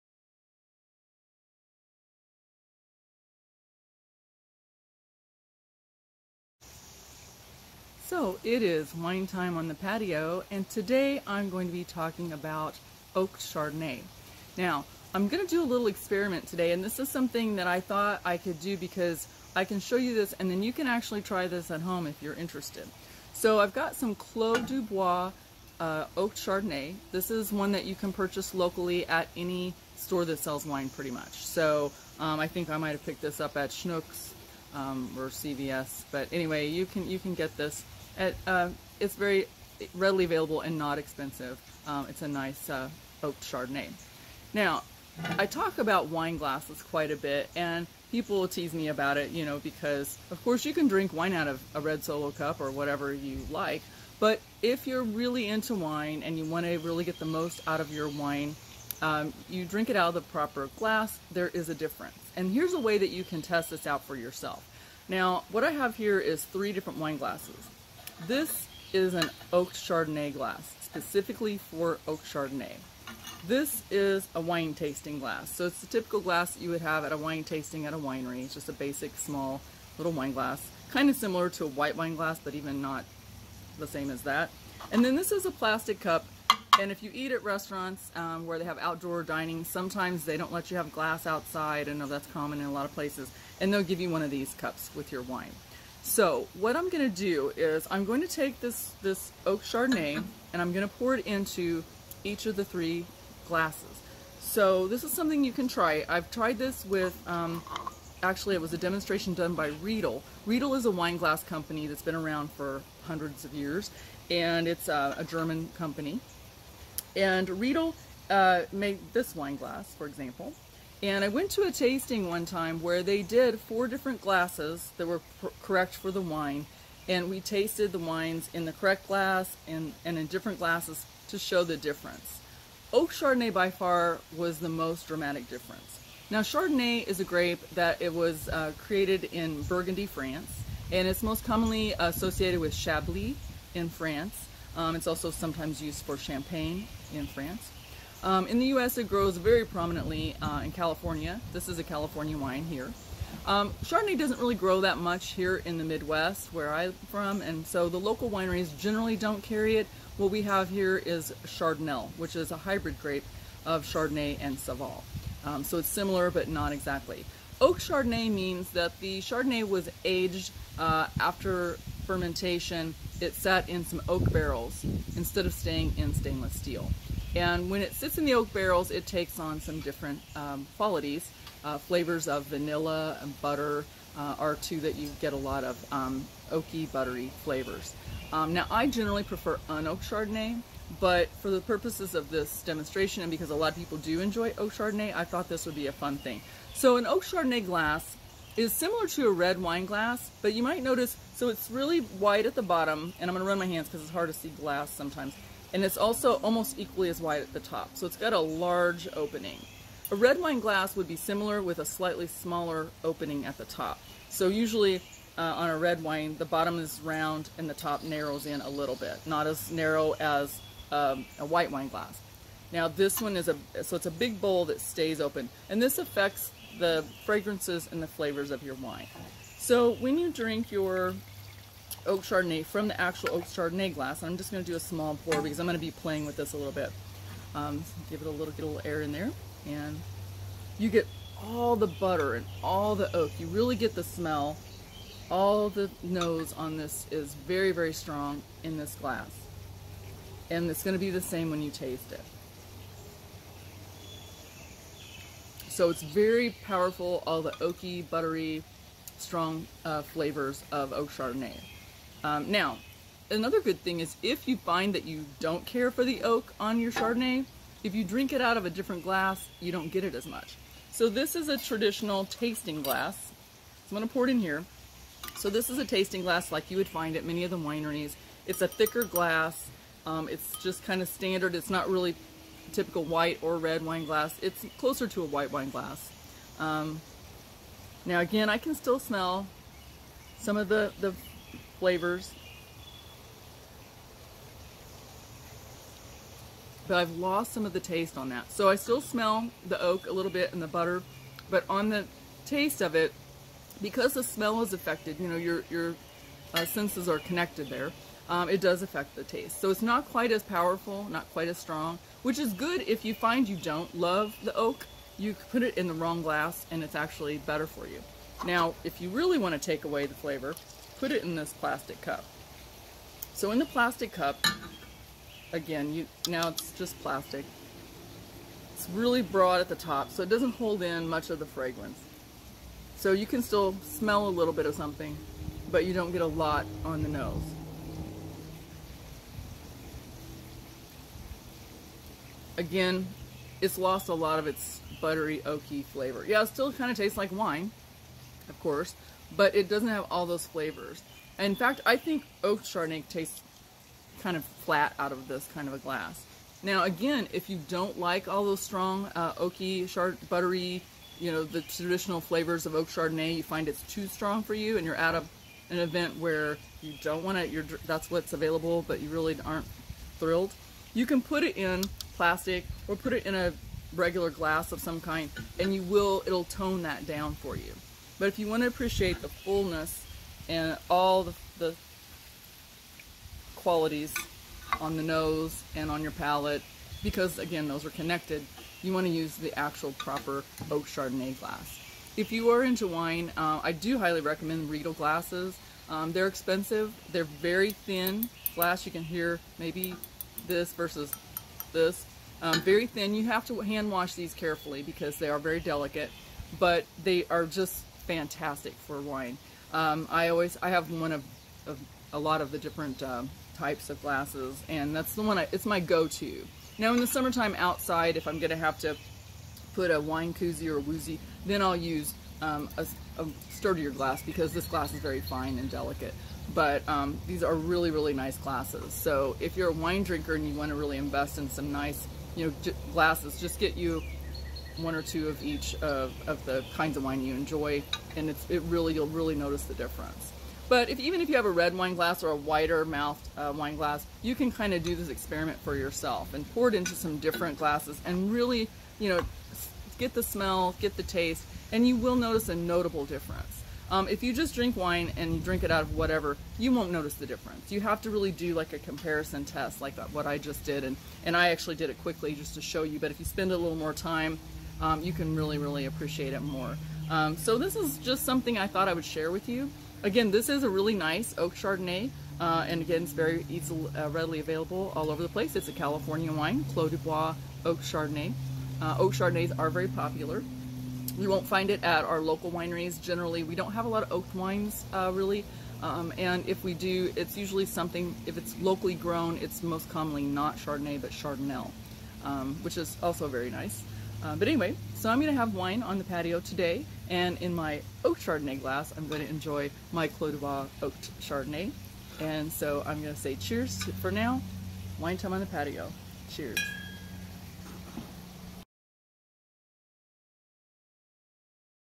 Wine Time on the Patio. Oaked Chardonnay Clos du Bois
In this video I share information on California Oaked Chardonnay from Clos du Bois, with a tutorial on choosing the proper wine glass.  I demonstrate a wine glass test that you can try at home to learn how much difference the proper wine glass can make to your enjoyment of various wines, in this case Oaked Chardonnay.